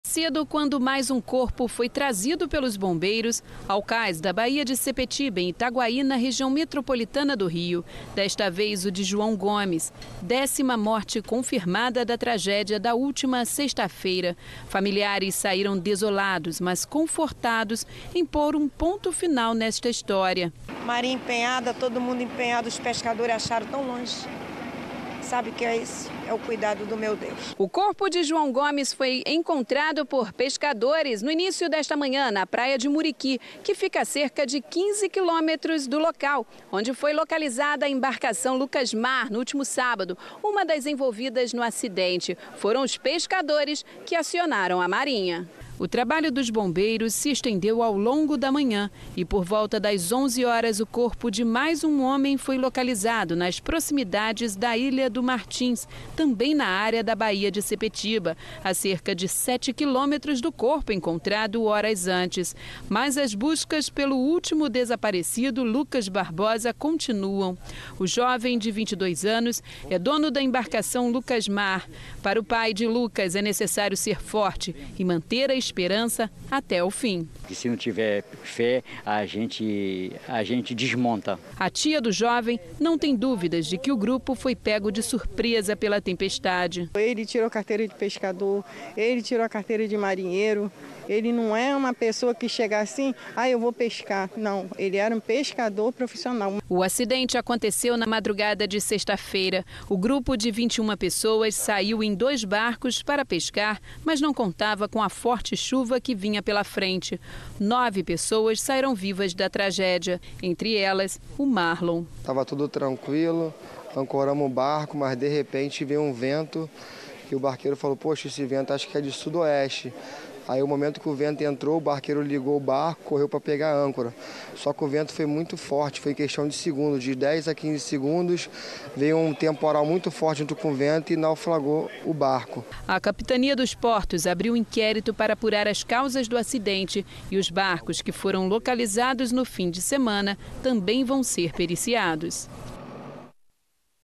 RJ: Bombeiros buscam pelo dono do naufrágio de Itaguaí
Os bombeiros continuam procurando pelo dono da embarcação que naufragou em Itaguaí, na região metropolitana do Rio de Janeiro.